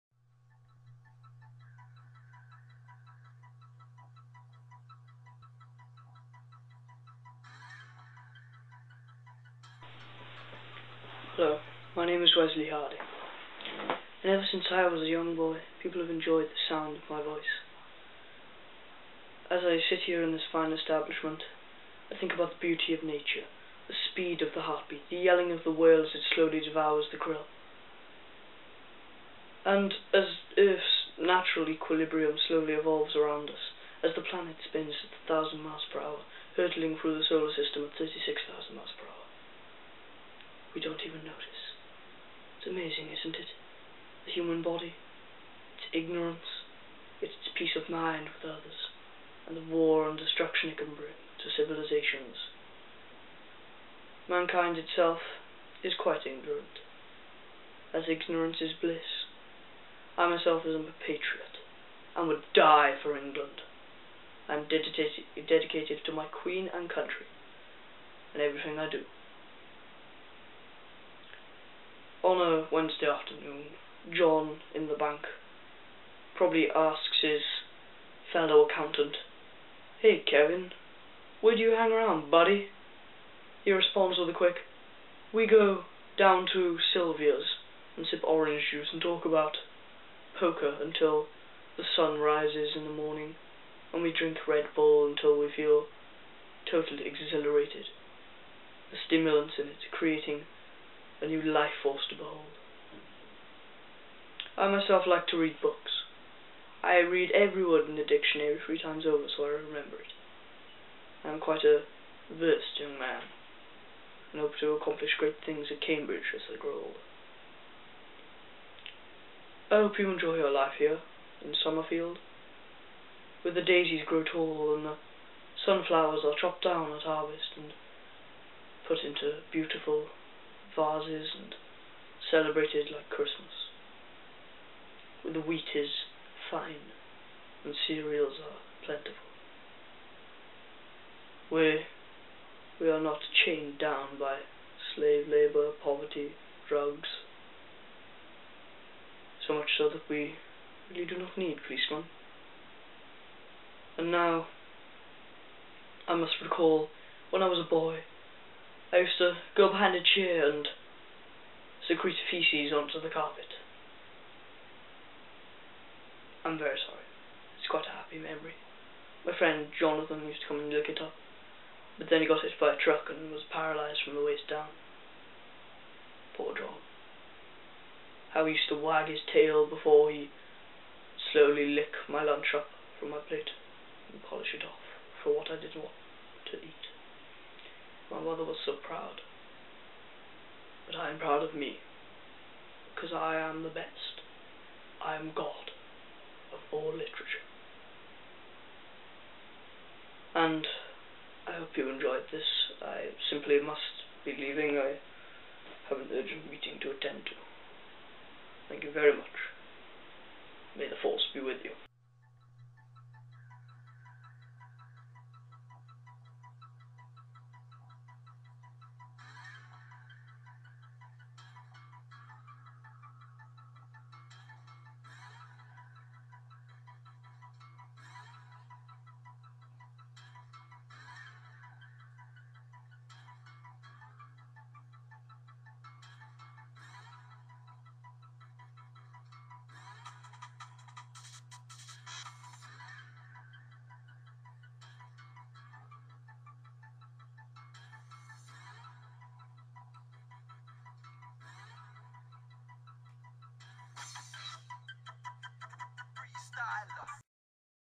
Kid On Drugs. By Wesley Harding.
watch wesley harding talk about life in his epic interview,,, it is 1 of those videos you just have to watch!